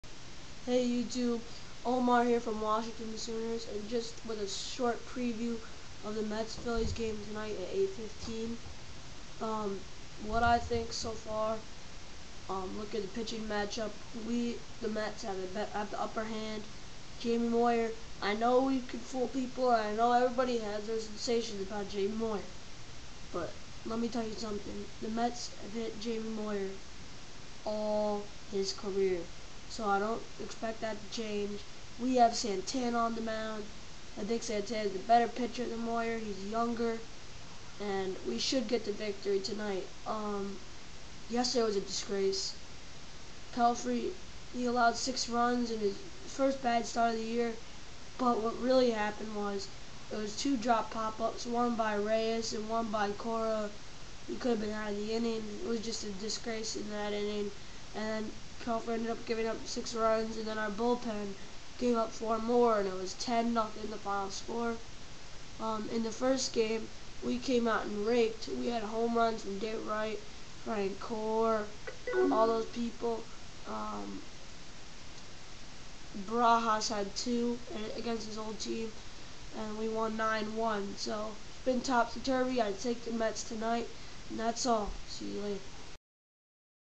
Mets@Phillies Preview
My short preview of tonights Mets Phillies game on 5/2/10 at 8:10